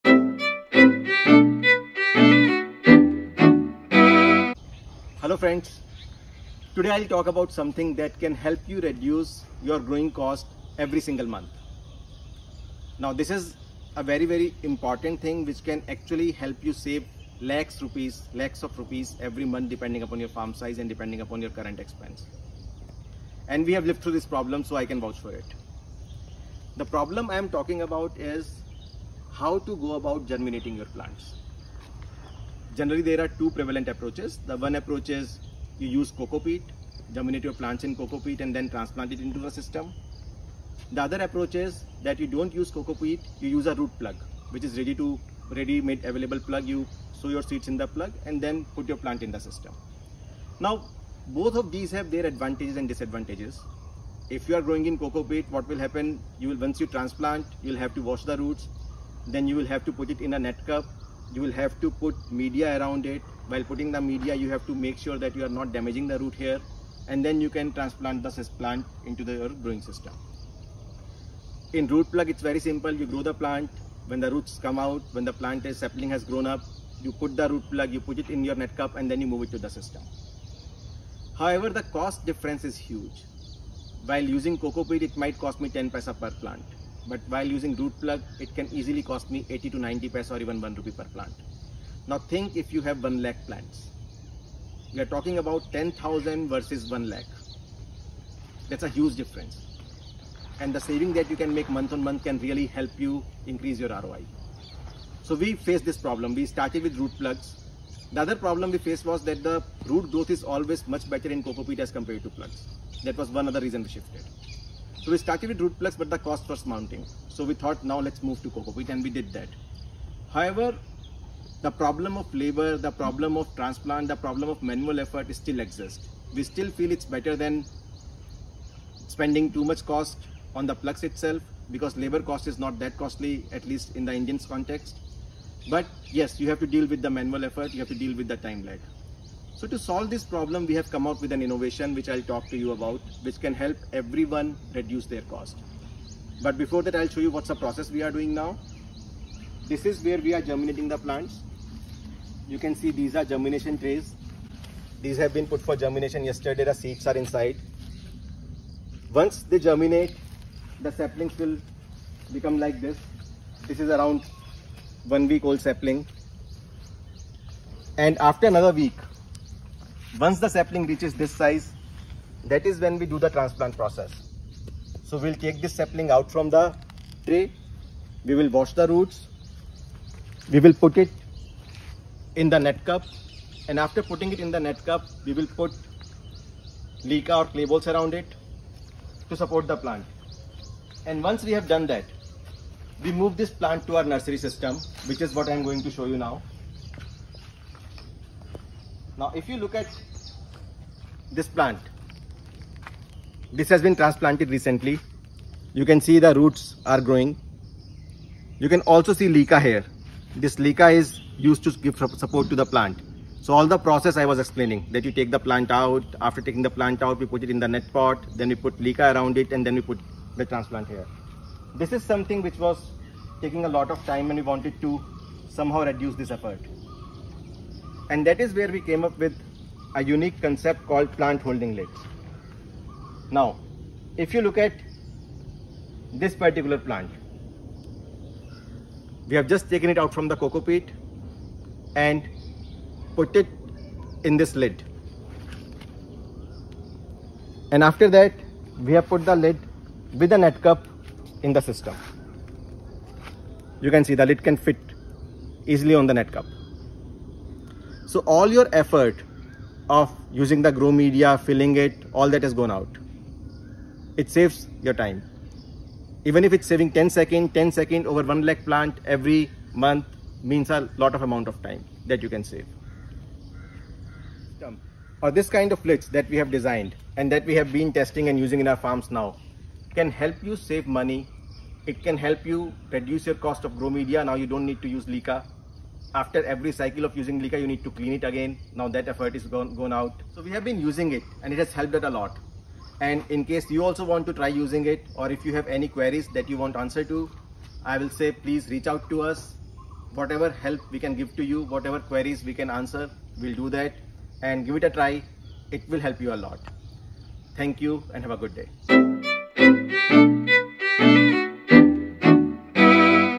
How to make transplant easy in a hydroponic farm and reduce monthly costs too?
One of the most laborious exercises in running a hydroponic farm is the transplantation of saplings into the growing system. Apart from labour, this process also involves the recurring costs of growing media and hidden costs caused by damage to plant roots and saplings too.

After struggling with this problem for a few years, we have finally come up with a simple, easy and effective solution that can be the game-changer for transplantation.

In this video, we will show you how you can save a lot of money and expenses on your hydroponic farm by using a simple invention that we have created to reduce our costs. Using hydroponic plant holding lids can result in guaranteed monthly savings for your farm. Understand how we are doing it. Now you can do it too.